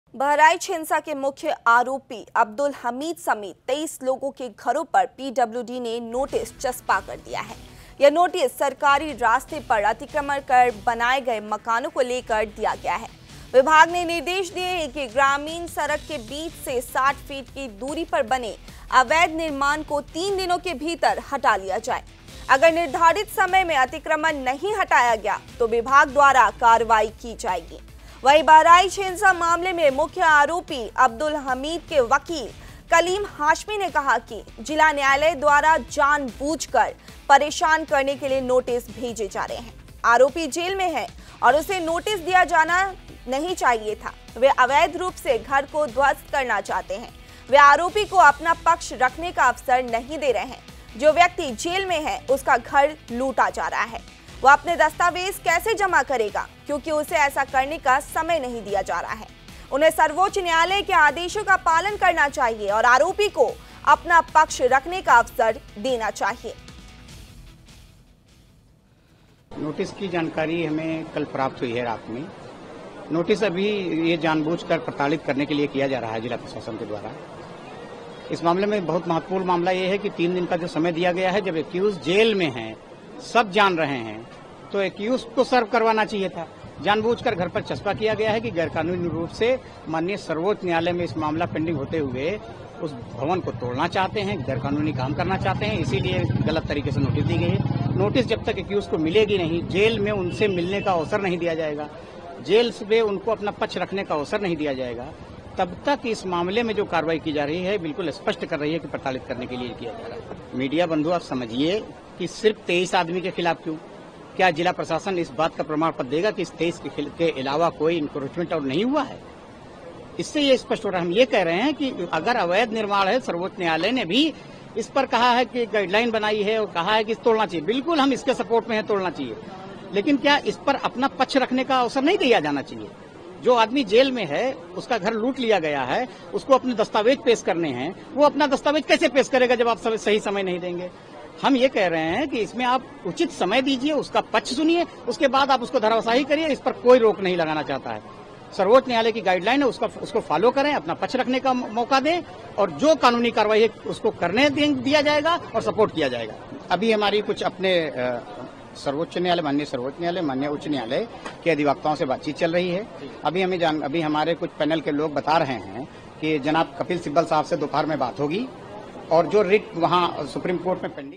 बहराइच हिंसा के मुख्य आरोपी पर गिरी गाज
पीडब्लुडीने दिया  नोटिस
23 लोगों के मकानों पर अतिक्रमण की नोटिसचस्पाई
तीन दिन में निर्माण हटाने का आदेश